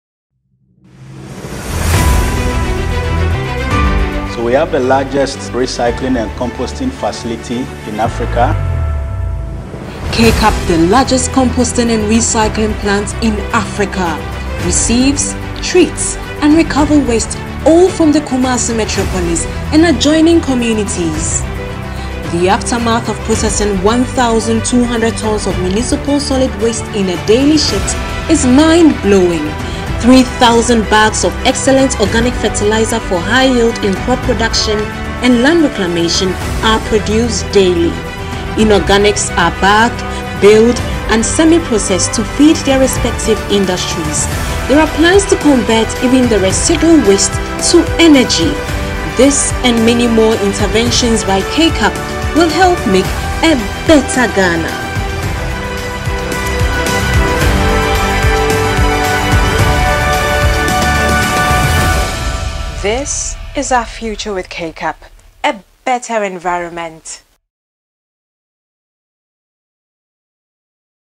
Welcome to KCARP
Discover how we turn mixed waste from the Kumasi Municipal and adjoining communities into raw materials for industry. We are able to produce highly organic compost, pelletized plastics and other recoverables with our groundbreaking technology here in Kumasi, Ghana.